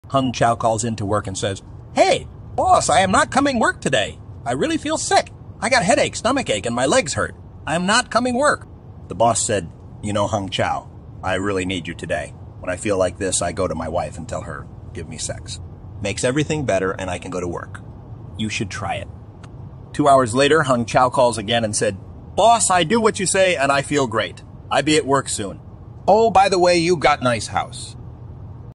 "HE'S ABOUT TO GET FIRED"
The Joke 🤣👇:

Hung Chow calls in to work and says: "Hey, boss I am not coming work today, I really feel sick. I got headache, stomach ache and my legs hurt. I am not coming work."
The boss said: "You know Hung Chow, I really need you today. When I feel like this I go to my wife and tell her give me sex.
Makes everything better and I can go to work.
Two hours later Hung Chow calls again and said: "Boss, I do what you say and I feel great. I be at work soon.
"Oh, by the way You got nice house."